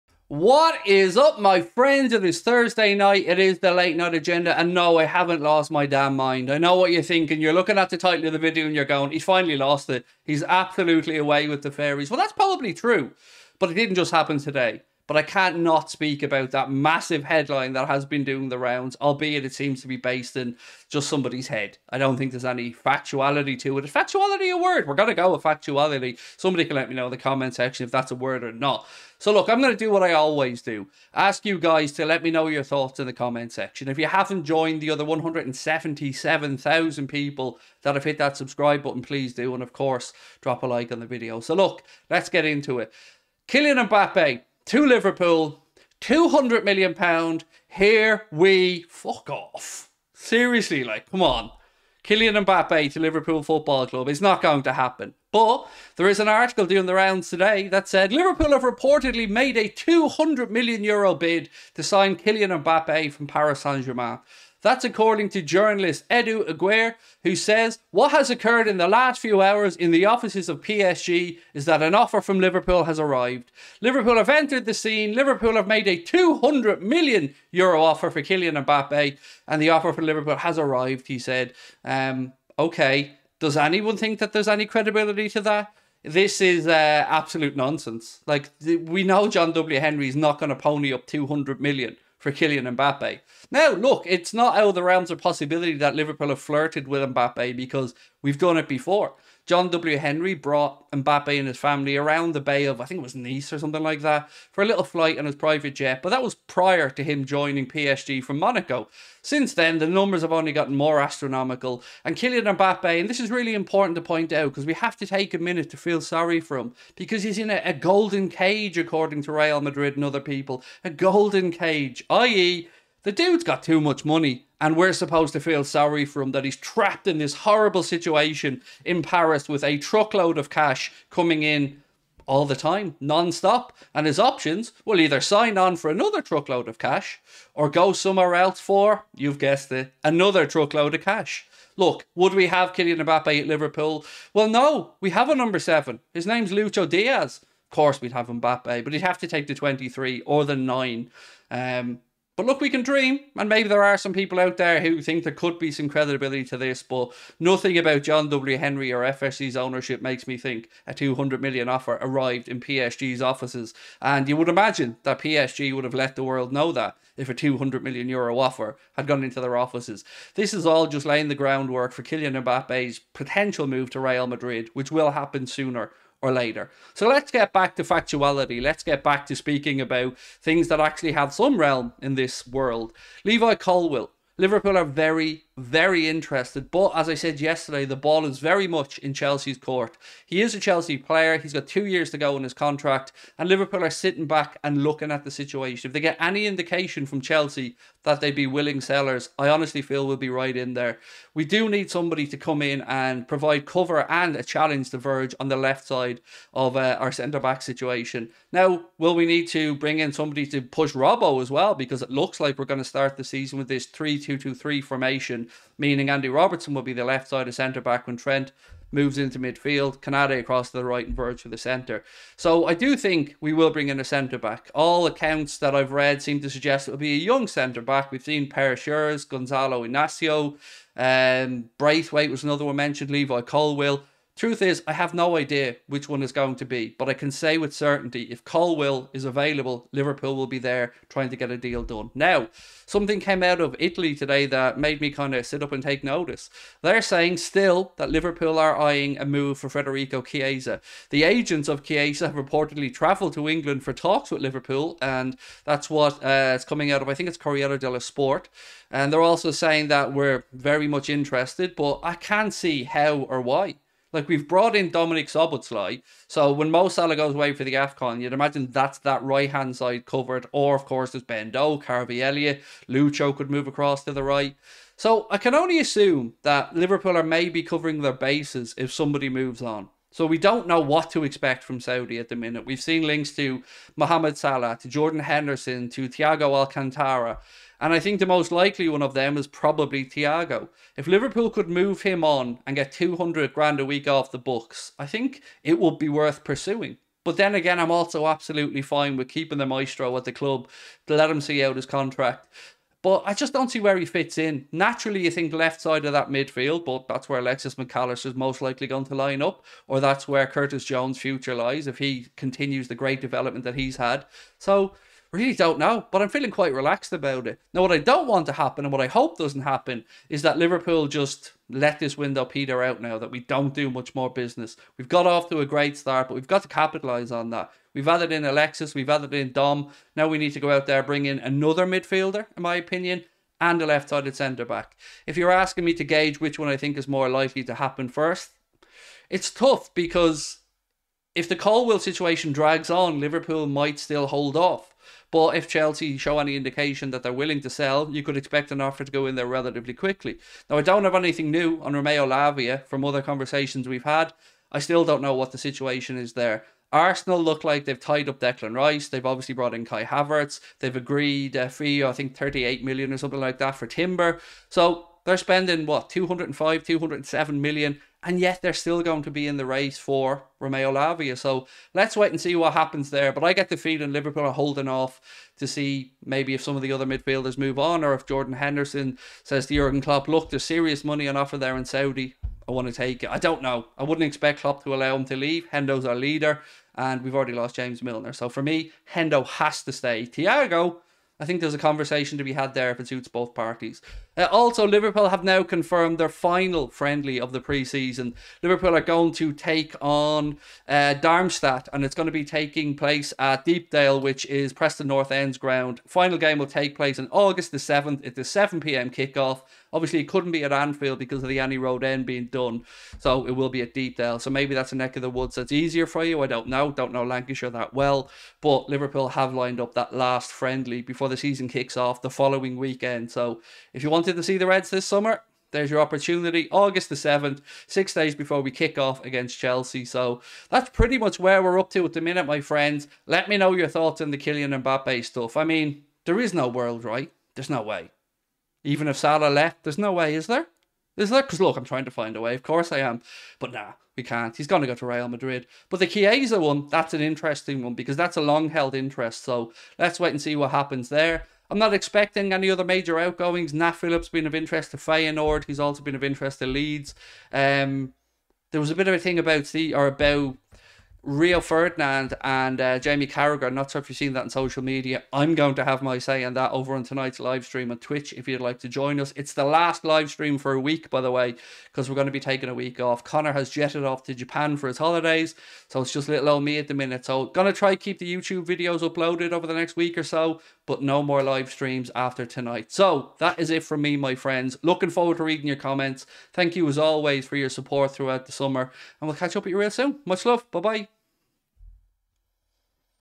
LIVERPOOL MAKE 200M MBAPPE OFFER TO PSG - REPORT CLAIMS
Craig Houlden reacts to the latest Liverpool news stories, including...

Listen to the full podcast on Spotify, Apple Podcasts, or just search 'Anfield Agenda' wherever you get your podcasts! 🎧

*Insert Title Here*